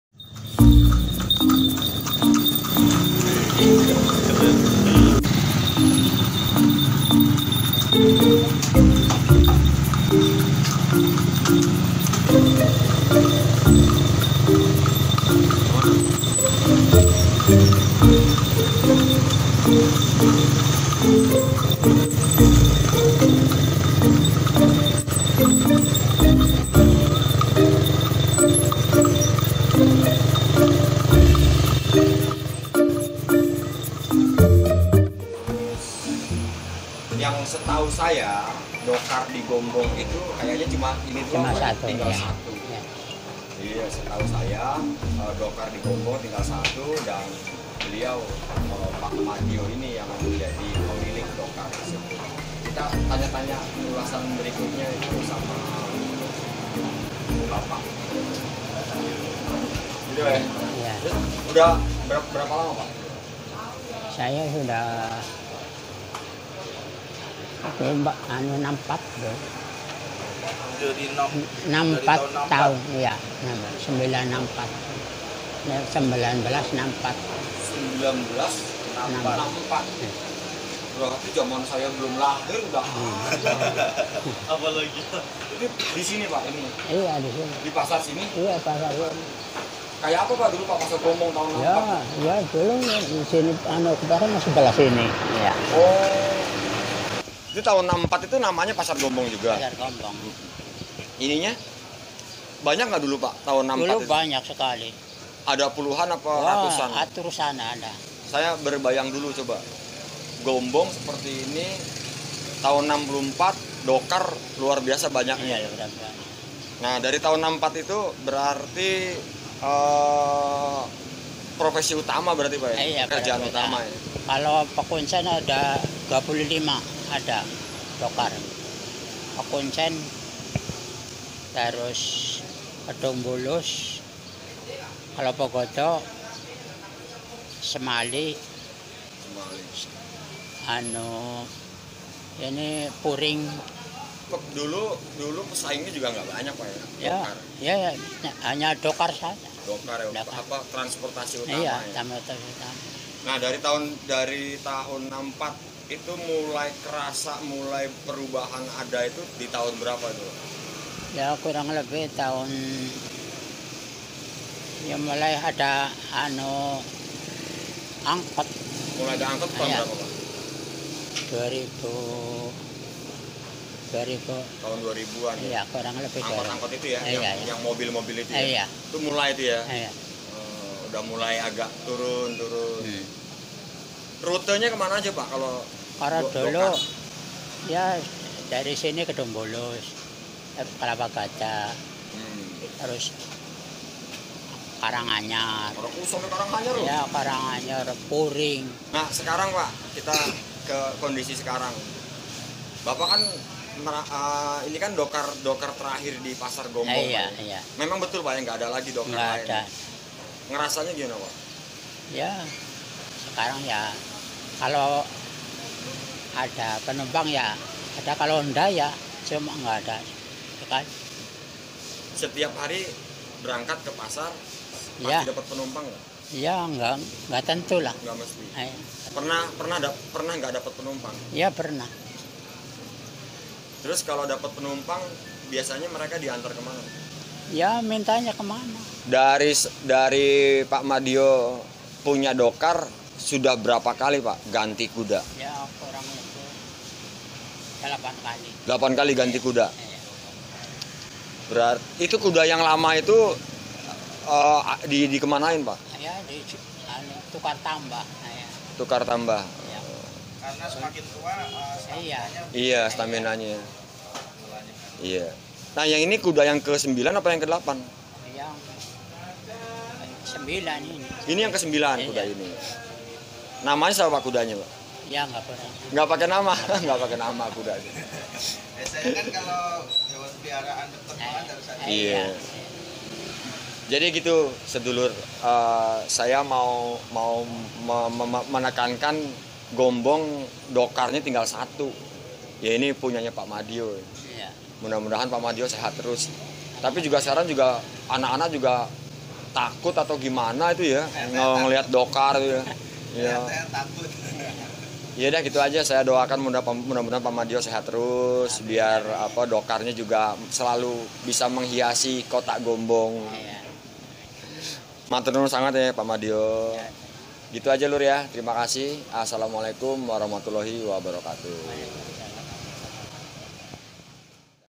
Dokar Terakhir Gombong
Pak Madyo , satu satunya kusir dokar yang masih ada di Gombong. Tiap hari mangkal di pasar gombong. Era transportasi yg semakin modern, membuat dokar hanya tinggal menunggu waktunya akan hilang...